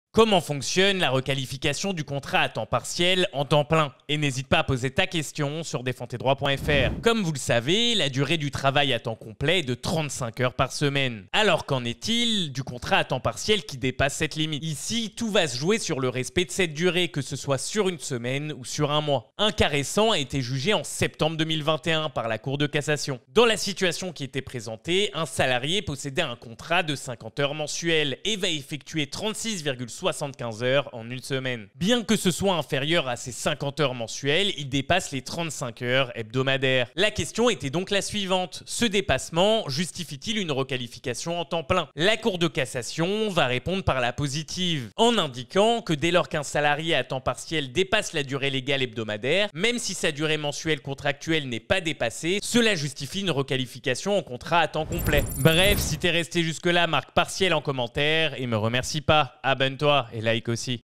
La requalification du contrat à temps partiel en temps plein ! Comment ça marche?
La requalification d'un contrat à temps partiel en contrat à temps plein est un processus juridique qui peut survenir dans certaines situations spécifiques. Dans cette vidéo, nous expliquons les circonstances dans lesquelles un contrat à temps partiel peut être requalifié en contrat à temps plein et comment cela fonctionne en France.

En général, la requalification peut se produire si les conditions du contrat à temps partiel ne sont pas conformes à la législation française, notamment en ce qui concerne la durée minimale de travail, les horaires, ou la rémunération. Les travailleurs à temps partiel ont des droits spécifiques en matière de protection sociale, de congés payés, et de rémunération, et il est essentiel que les employeurs respectent ces droits.

Si un contrat à temps partiel est requalifié en contrat à temps plein, cela peut avoir des implications importantes pour l'employeur en termes de cotisations sociales et d'avantages sociaux pour le travailleur.

Pour plus d'informations sur la requalification des contrats à temps partiel en contrats à temps plein, consultez notre ou contactez-nous au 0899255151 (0,80 ctm la minute).